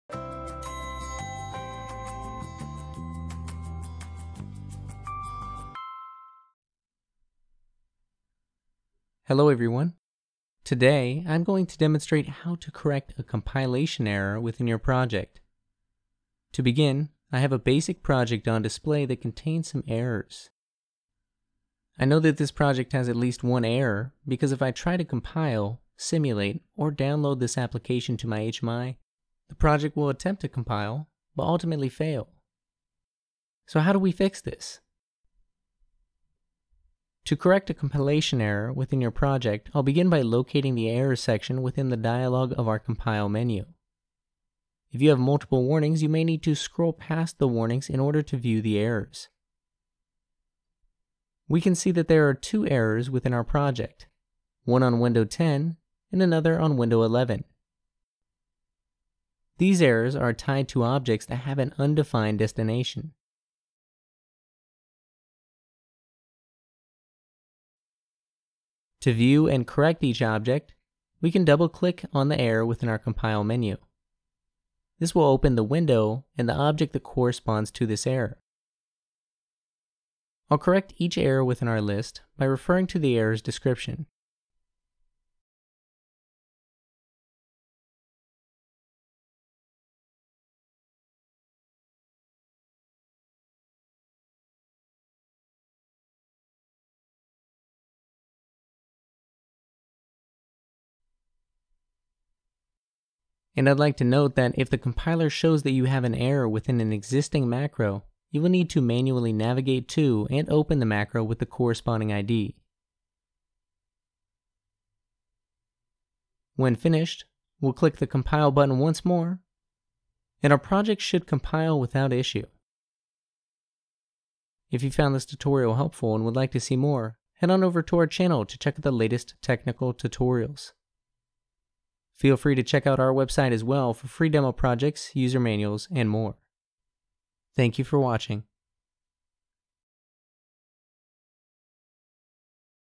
How to correct a compilation error - EasyBuilder Pro
Within this tutorial, we demonstrate how to correct a compilation error using the compile dialog within EasyBuilder Pro.

0:00 - Introduction
0:35 - Locating errors
1:19 - Correcting compilation errors
1:40 - Macro compilation errors